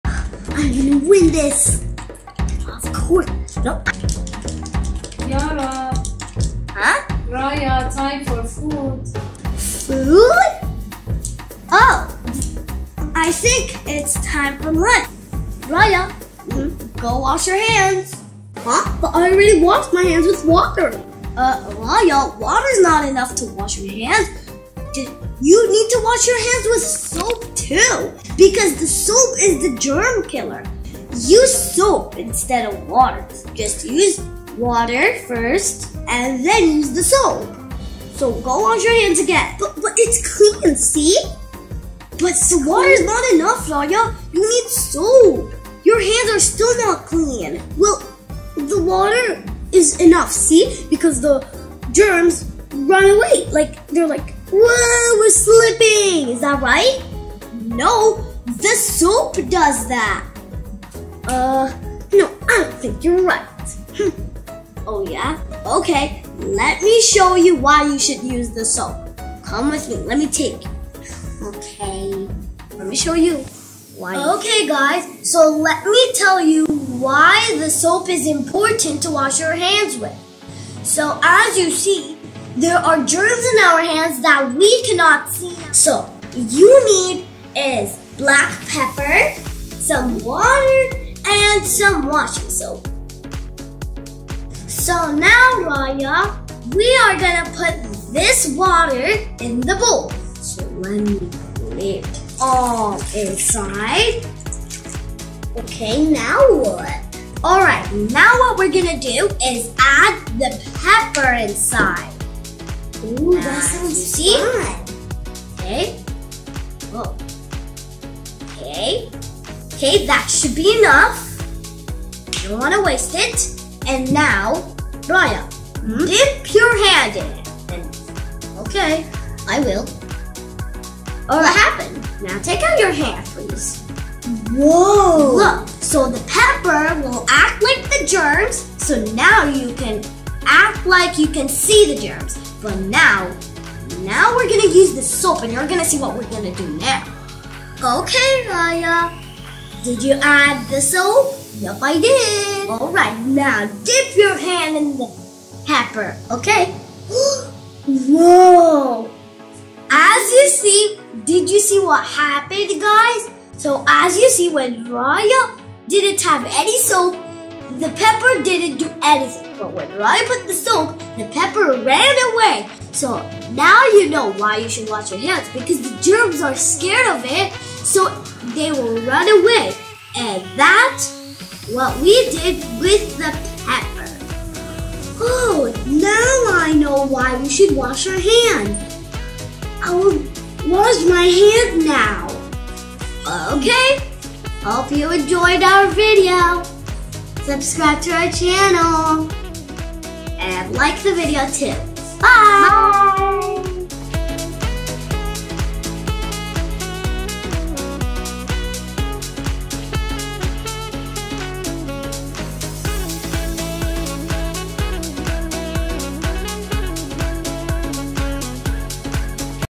Pepper and Soap Science Experiment for Kids | Why it is Important to Wash Hands with Soap?
Hi!! In this video we show you why it is important to wash hands with soap :)